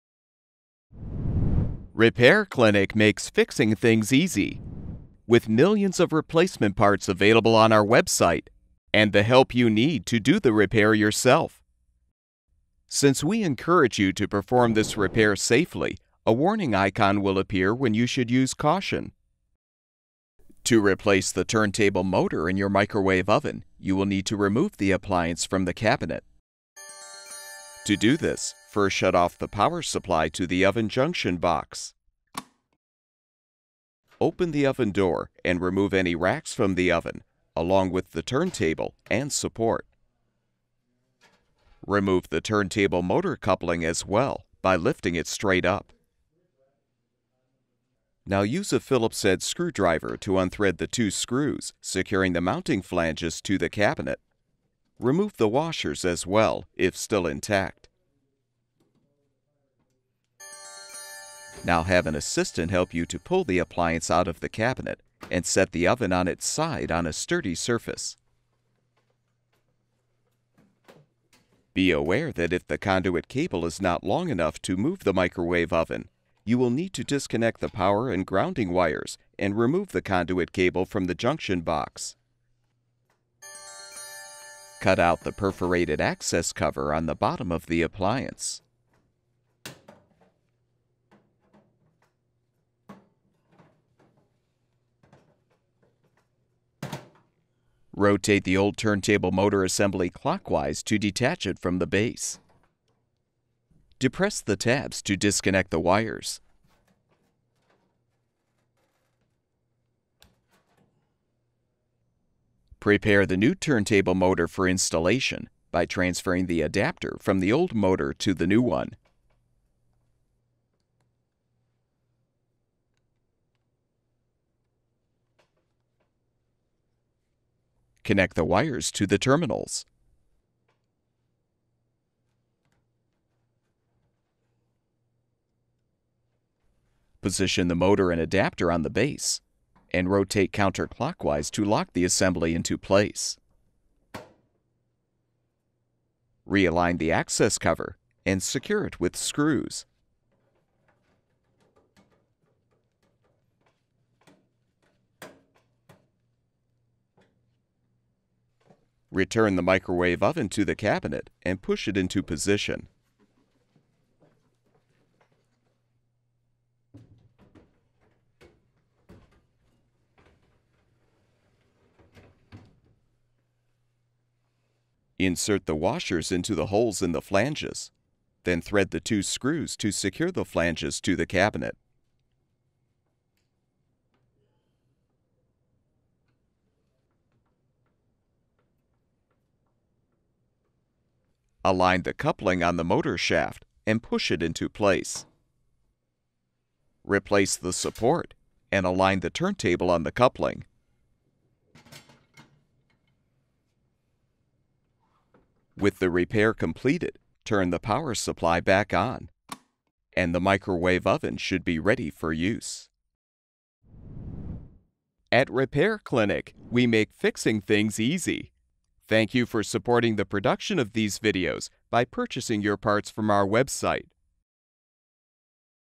KitchenAid Microwave Turntable Motor Replacement W10911403
This video provides step-by-step repair instructions for replacing the turntable motor on a KitchenAid microwave (Model KMBP100ESS01). The most common reason for replacing the motor is if the turntable will not rotate.

All of the information for this microwave turntable motor replacement video is applicable to the following brands:
KitchenAid, Whirlpool

Tools used: multi-screwdriver